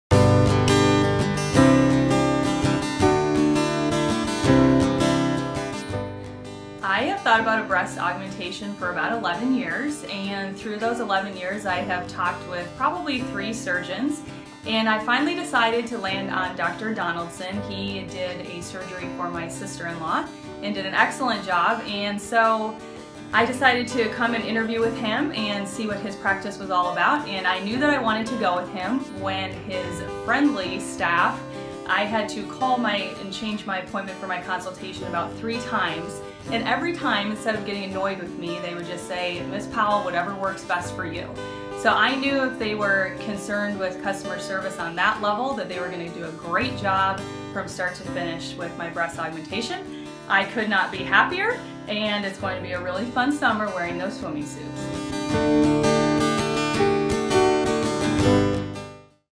Columbus, Ohio Breast Augmentation Patient Says, "It's Going to be a Really Fun Summer!"
This patient interviewed three plastic surgeons over a span of eleven years before choosing Dr. Donaldson for her breast augmentation surgery.  She's glad she finally made the decision, and she raves about the customer service throughout her entire experience at Donaldson Plastic Surgery.

"I have thought about breast augmentation for about 11 years. Through those 11 years, I have talked with probably about 3 surgeons and I finally decided to land on Dr. Donaldson.  He did a surgery for my sister-in-law and did an excellent job and so I decided to come and interview with him and see what his practice was all about.  I knew that I wanted to go with him when his friendly staff--I had to call and change my appointment for my consultation about 3 times--and every time instead of getting annoyed with me, they would just say, “Ms. Powell, whatever works best for you!”.  So I knew they were concerned with customer service on that level; that they were going to do a great job from start to finish with my breast augmentation.  I could not be happier and it’s going to be a really fun summer wearing those swimming suits!”